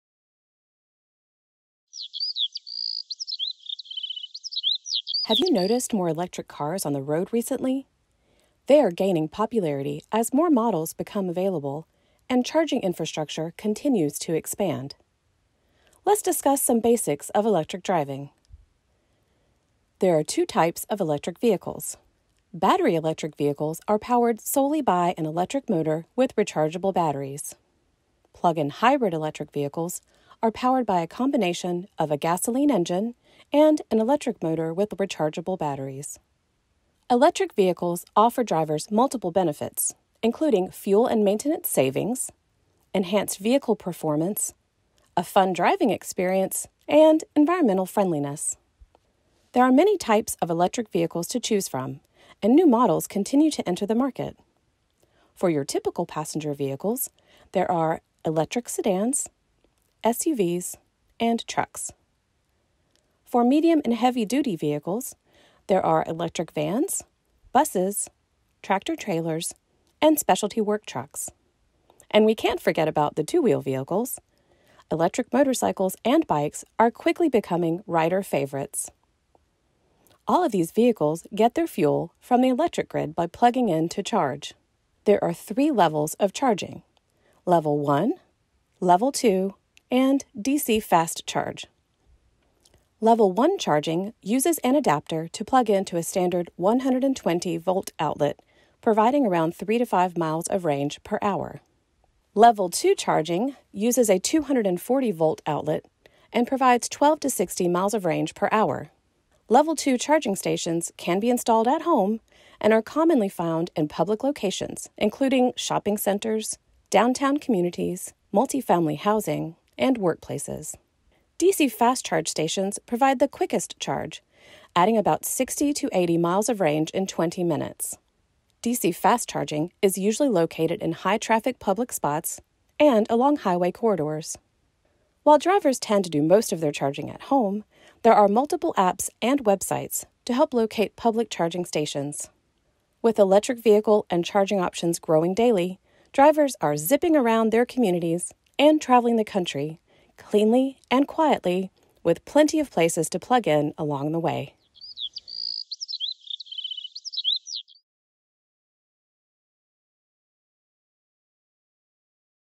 Introduction to Electric Vehicles
This video provides an overview of electric vehicles and charging stations. It was produced by our electric vehicle program, Plug-in NC